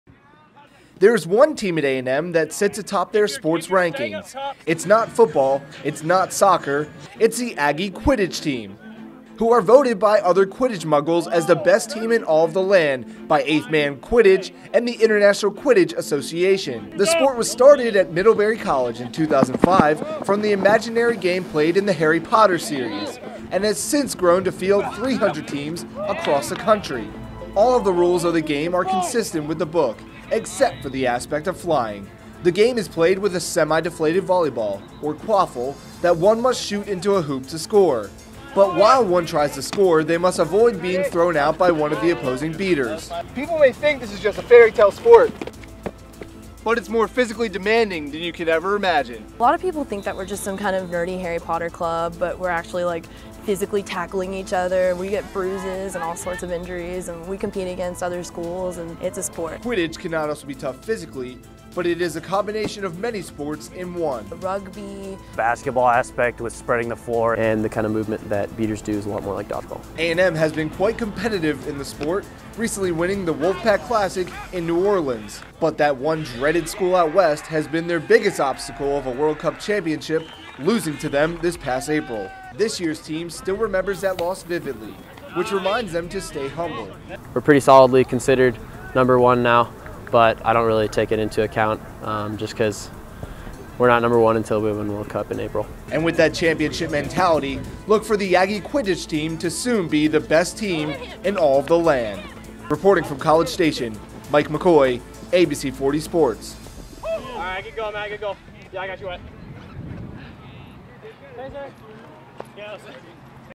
Texas A&M Quidditch
They are the number one team in the country and are not just a "Nerdy Harry Potter Fan Club." This sport involves tackling and strong physical endurance to excel in this league and the Aggies want to avenge their loss to the Longhorns from last year!